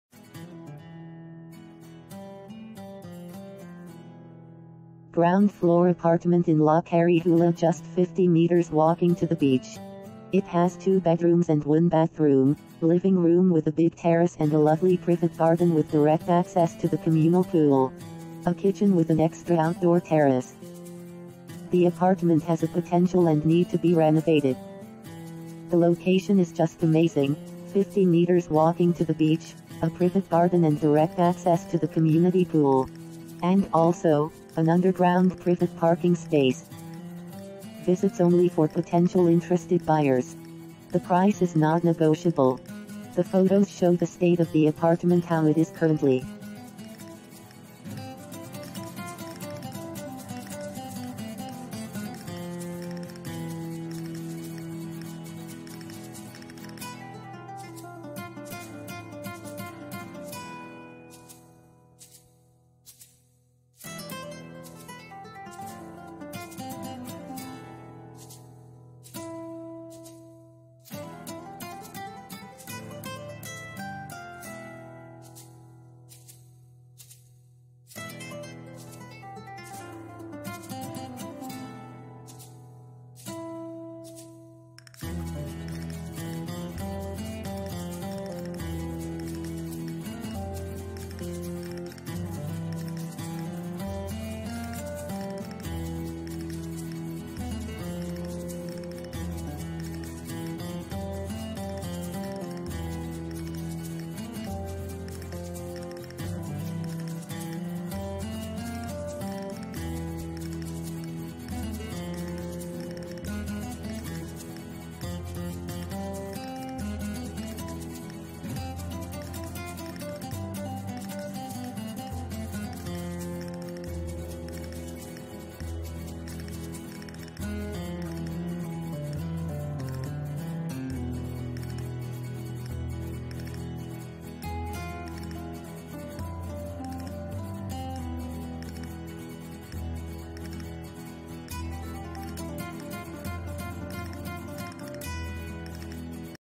2 Bedrooms Ground Floor Apartment in La Carihuela, Costa Del Sol FOR SALE &euro; 299,000
2 Bedrooms Ground Floor Apartment in La Carihuela, FOR SALE &euro; 299,000
Ground floor apartment in La Carihuela just 50 meters walking to the beach. It has 2 bedrooms and 1 bathroom, living room with a big terrace and a lovely privet garden with direct access to the communal pool. A kitchen with an extra outdoor terrace.

The apartment has a potential and need to be renovated.

The location is just amazing, 50 meters walking to the beach, a privet garden and direct access to the community pool. And also, an underground privet PARKING space.

Visits only for potential interested buyers. The price is not negotiable. The photos show the state of the apartment how it is currently.
Fitted Wardrobes;Private Terrace
Beachfront;Commercial Area;Beachside;Close To Shops;Close To Sea;Front Line Beach Complex